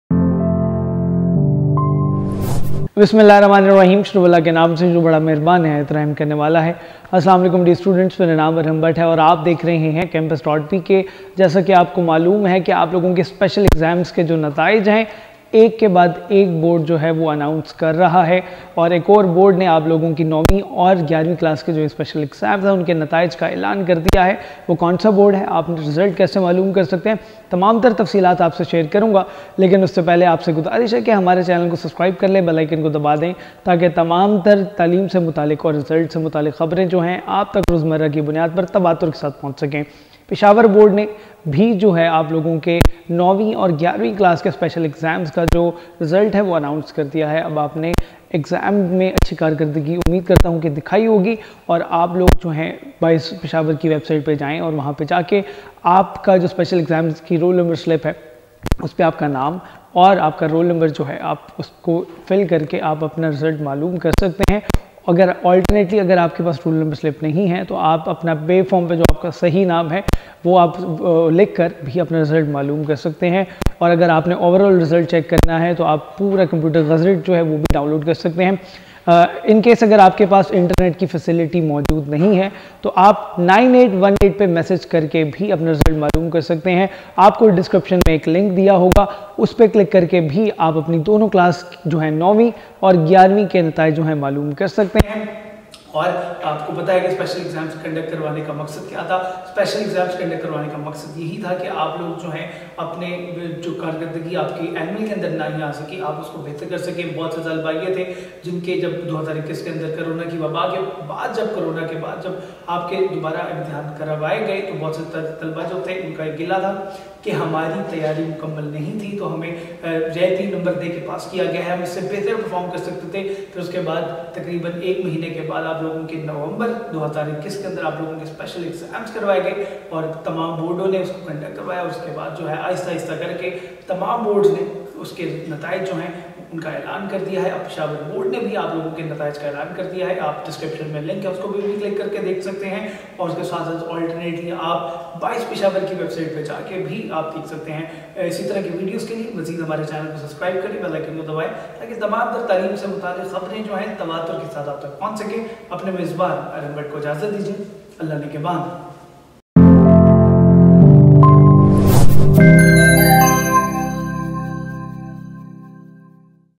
Bise Peshawar Board 9th & 11th Class Special Exams 2021 Result
BISE Peshawar board has announced 9th and 11th Second Annual Examination 2021 result. The students of 9th class were waiting for the 9th class result that were appeared in Special exams. SSC pat 1 result can be seen online on the official website of the board of intermediate and secondary education Peshawar. Students’ appeared in the exams of 11 class can check their 11th class result online.
For further Queries and Latest Educational news you can follow our different social media handles